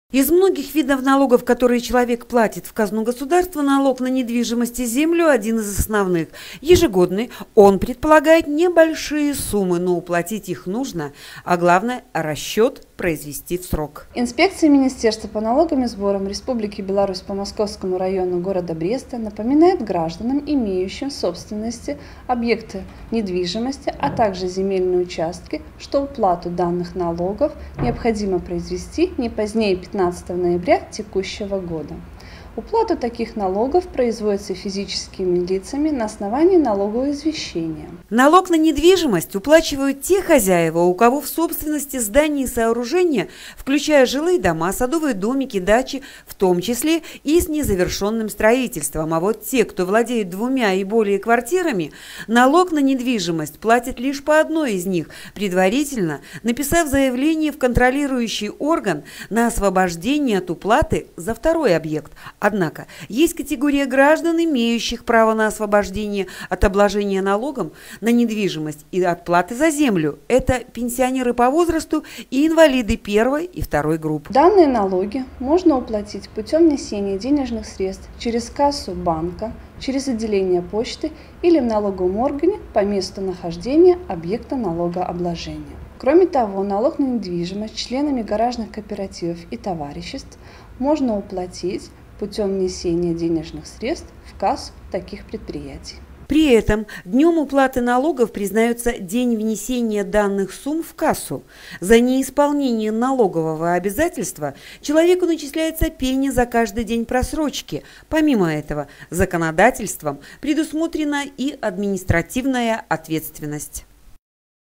2013-08-26 г. Брест Телекомпания "Буг-ТВ". Имущественные налоги: срок уплаты до 15 ноября.
Журналист Л. Осмолович. Оператор В. Холявко
Именно налоги являются источником доходной части бюджетов всех уровней любого государства. Белорусы также принимают участие в формировании бюджета  путем уплаты пошлин, налогов и иных платежей. Один из основных - налог на недвижимость и землю.
 По статистике их платит каждый третий житель страны. В налоговой инспекции по Московскому району города Бреста более 18 тысяч человек вносят в казну государства отчисления по этому виду налога. О некоторых нюансах расчета за землю и недвижимость расскажет главный налоговый инспектор отдела консультаций  налоговой инспекции по Московскому району города Бреста Оксана Боровенко.